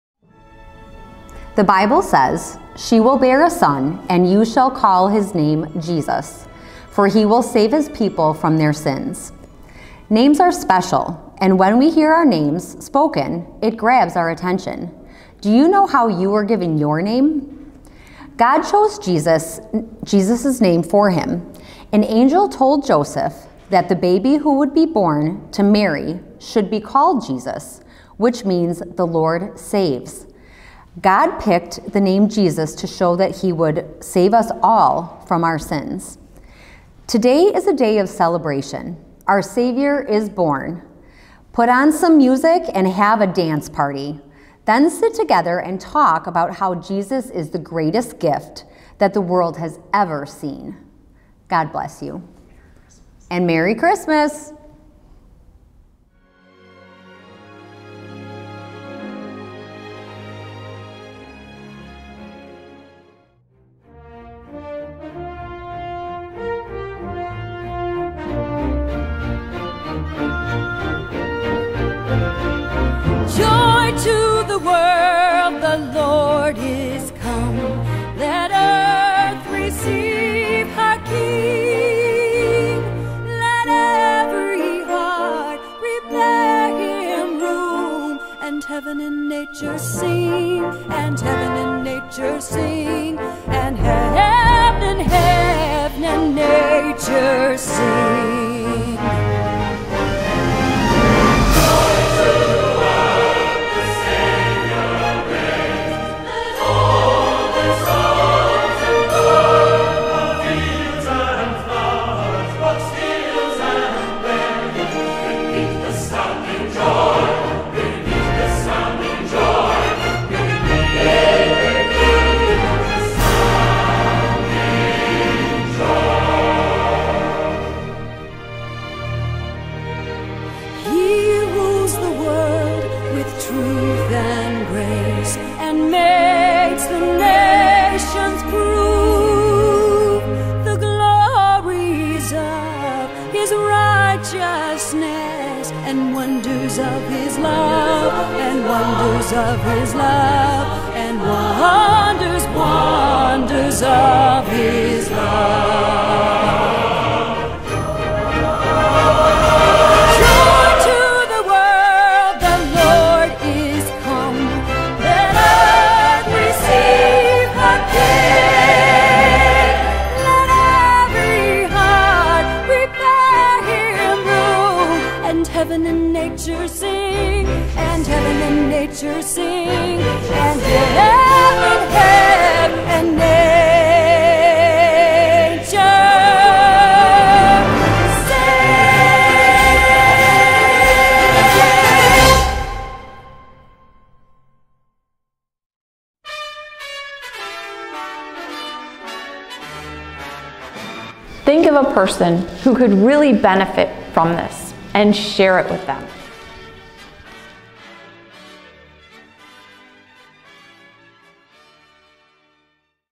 Colleen Gallo - Trinity's Advent Devotion
Colleen Gallo shares Trinity's Advent Devotion.
Take the next steps in discipleship